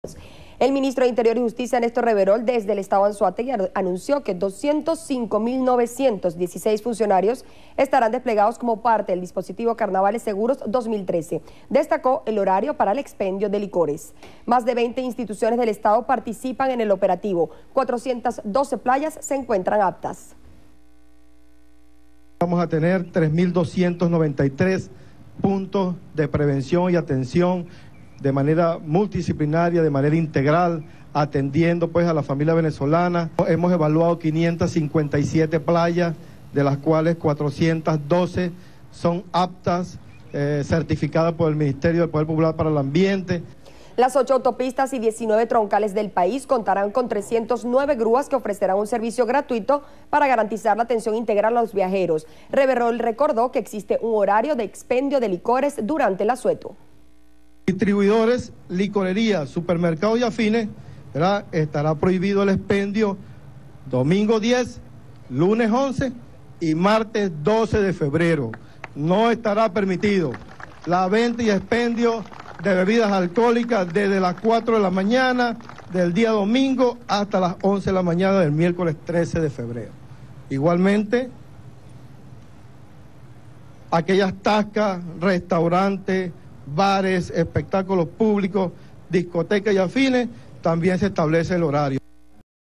Más de 200 mil funcionarios velarán por la seguridad de los temporadistas en Carnaval
Suscríbete El ministro de Interior y Justicia, Néstor Reverol, desde el estado Anzoátegui, anunció que 205.916 funcionarios estarán desplegados como parte del dispositivo "Carnavales seguros 2013" para el resguardo de la seguridad de los temporadistas. Reverol destacó que más de 20 instituciones del estado participan en el operativo, a la vez que anunció que 412 playas se encuentran aptas.